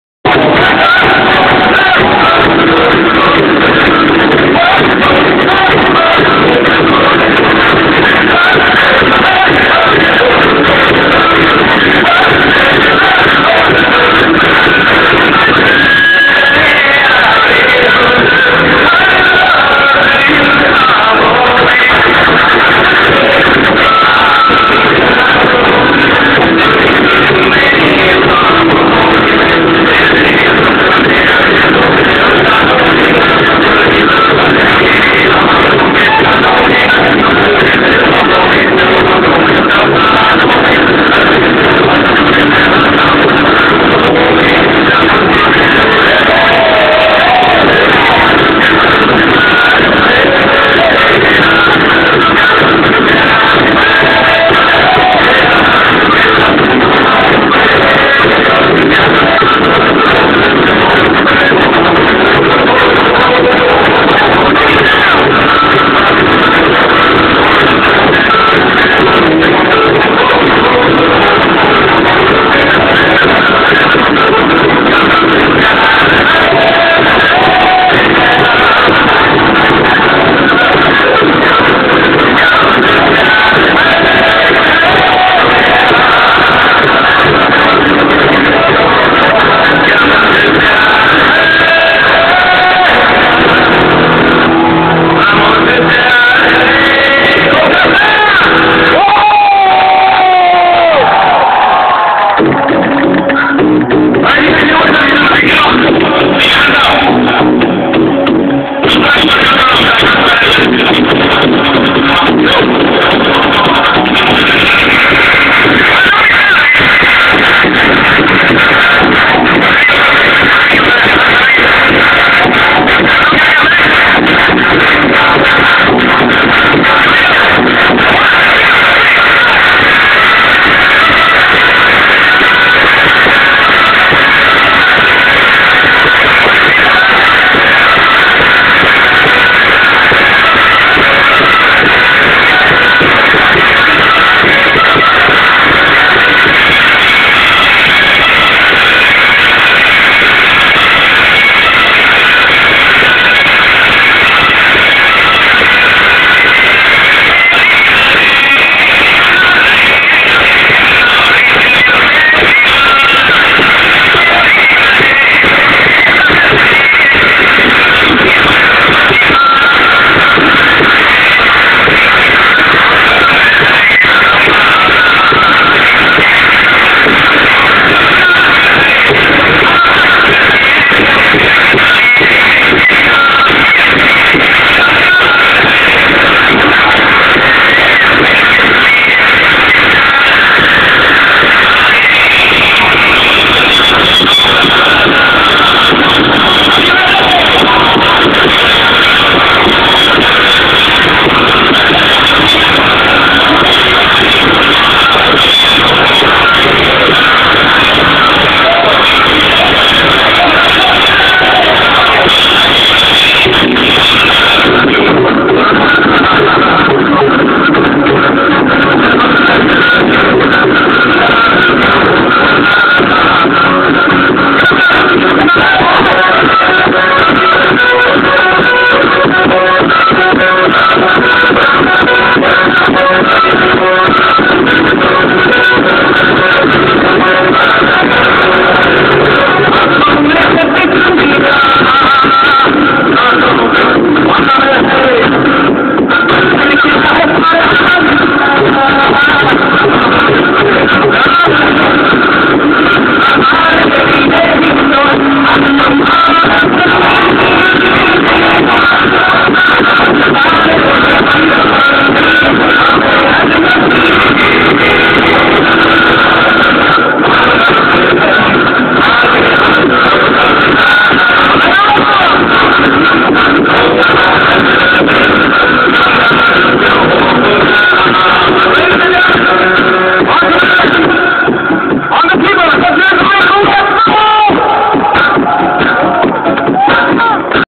KK - Beyond Barriers IX 2008
KK live at SXCAA's Beyond Barrier's Chapter IX, 28th Jan 2008